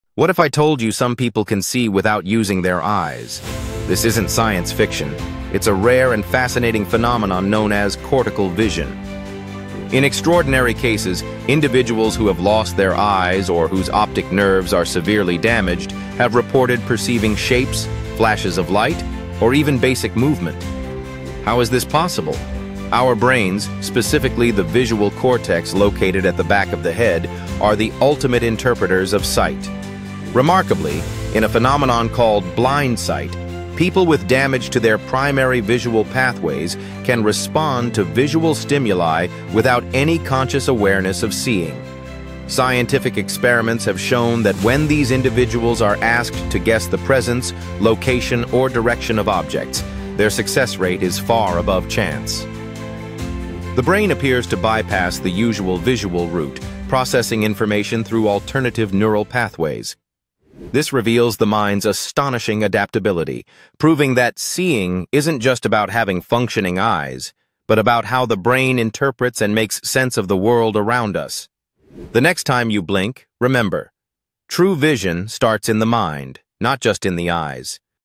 Seeing Without Eyes: The Brain's Hidden Vision
Did you know some humans can 'see' without eyes? Discover the fascinating truth about the brain's visual powers!